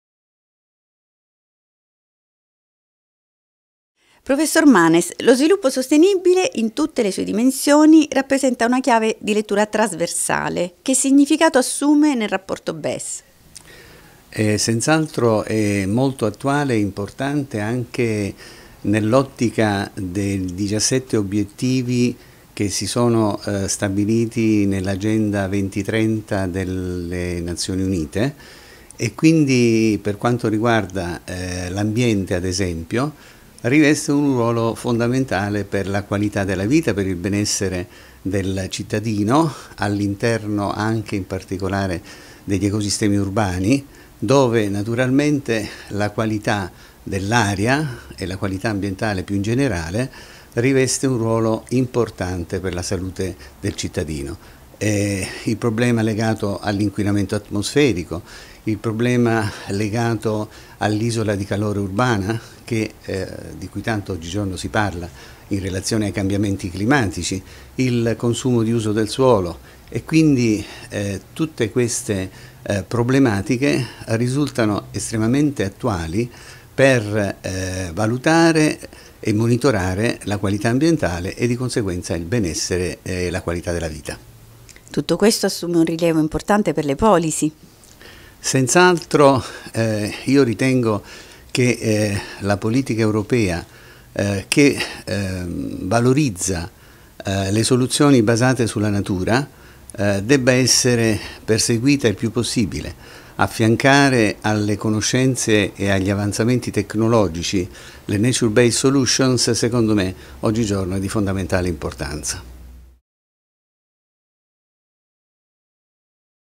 Il significato dello sviluppo sostenibile nel Rapporto BES. Fausto Manes Sapienza Università di Roma
La valorizzazione delle infrastrutture verdi comporta il miglioramento della qualità di vita nelle aree metropolitane.
Investendo nelle aree verdi è possibile migliorare qualità ambientale, del territorio e, di conseguenza, il benessere del cittadino. Tutto ciò ha un ruolo importante per le politiche europee.